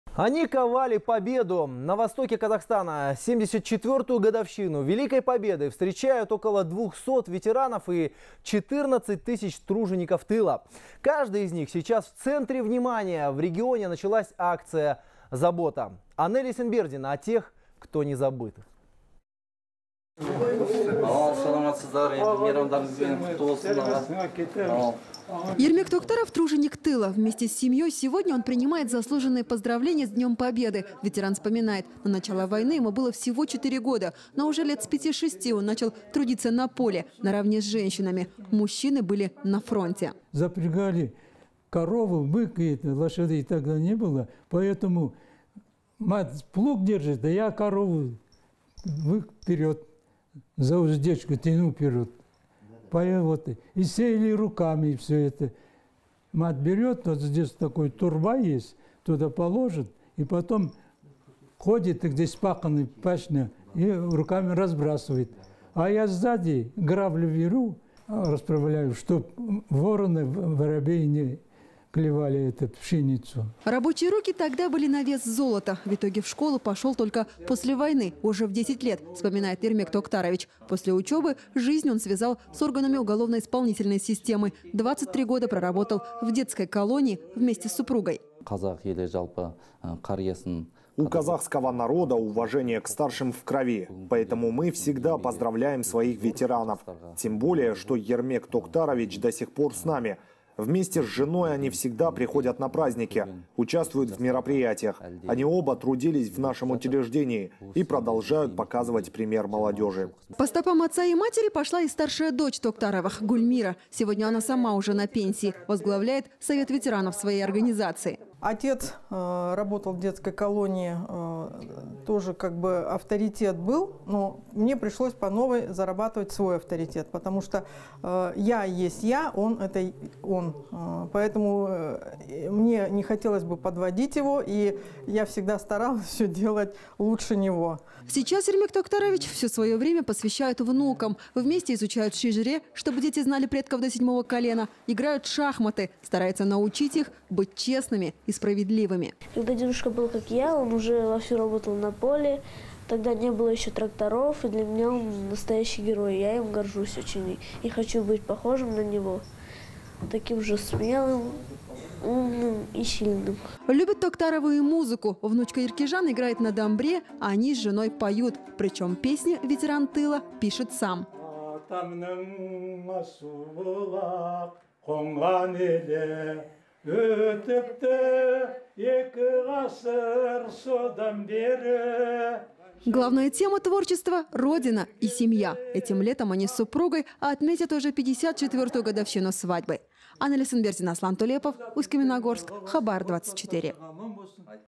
Труженики тыла начинали работать с 5-6 лет
На востоке Казахстана 74-ю годовщину Великой победы встречают около 200 ветеранов и 14 тысяч тружеников тыла. Каждый из них сейчас - в центре внимания. В регионе началась акция «Забота», сообщает корреспондент «Хабар 24».